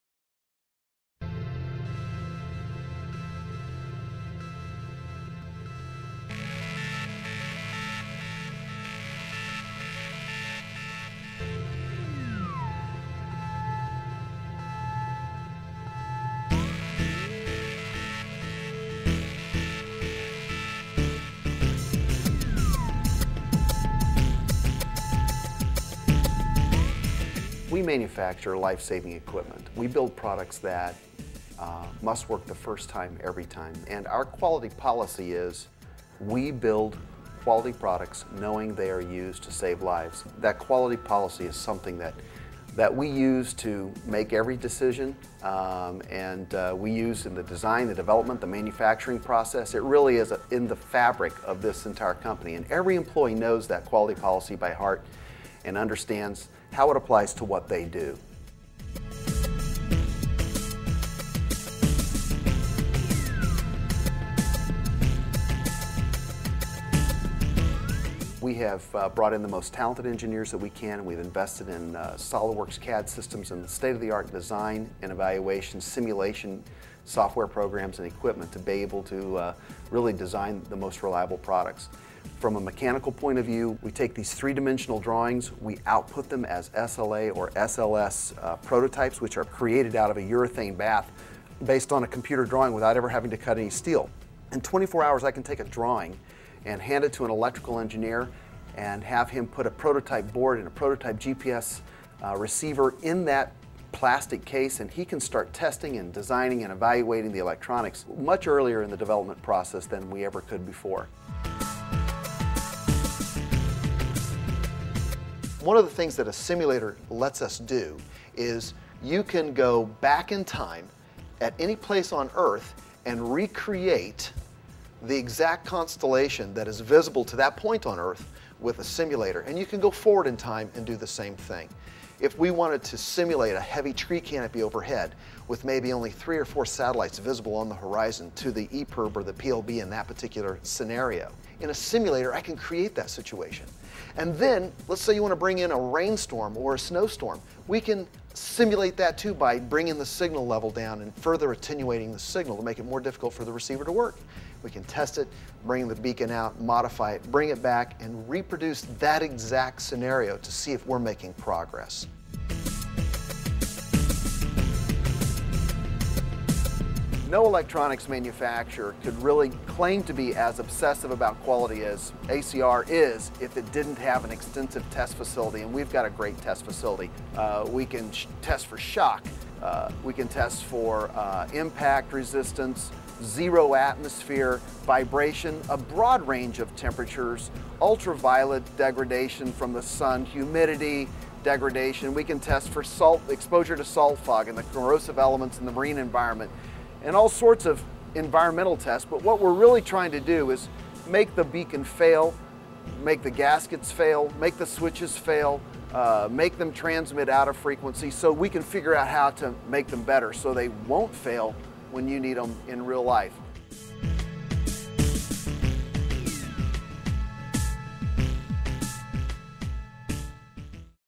ACR Factory Tour - Design and Validation
In Part one of our factory tour, learn how ACR goes above and beyond when designing new products, testing the design and validating that any of our new products will withstand anything mother nature can throw at them.